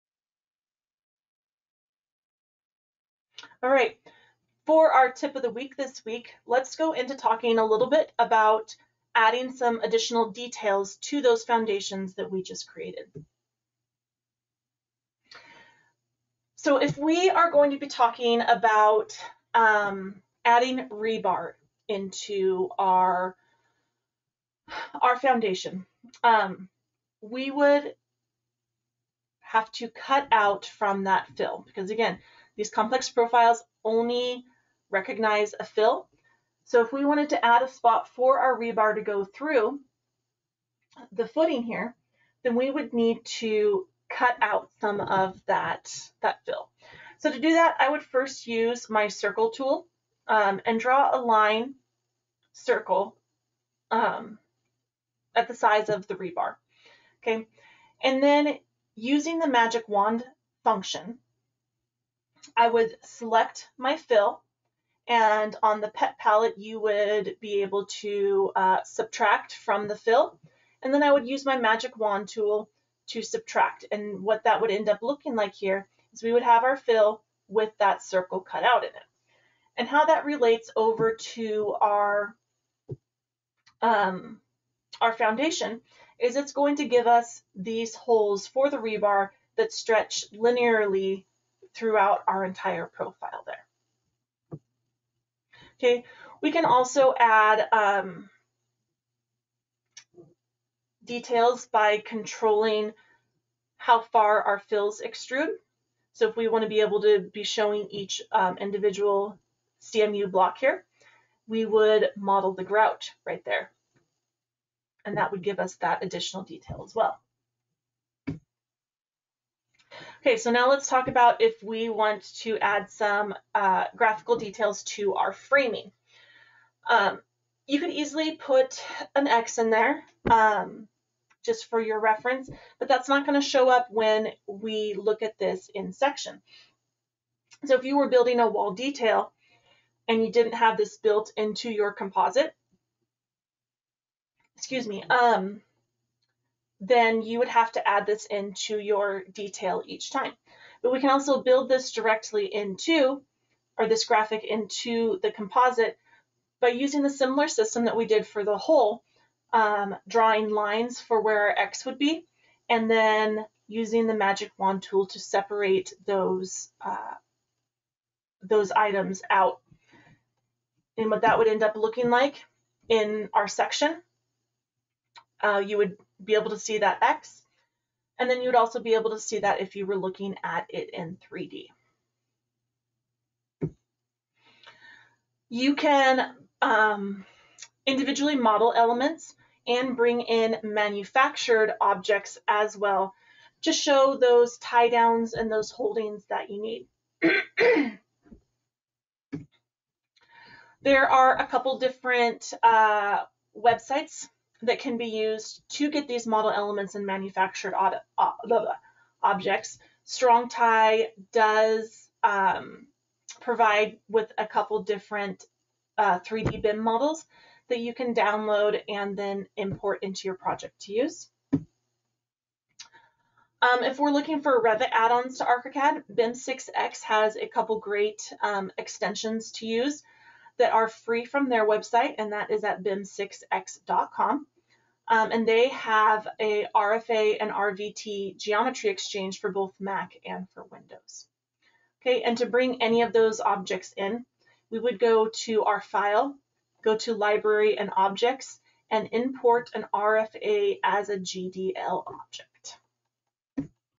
Adding Details to the Foundation
The complex profile editor allows us to only add elements once we have cut out spaces for them. In this tip of the week, we will walk you through creating details with the editor toolbox for adding rebar, framing graphics, and modeled elements and objects.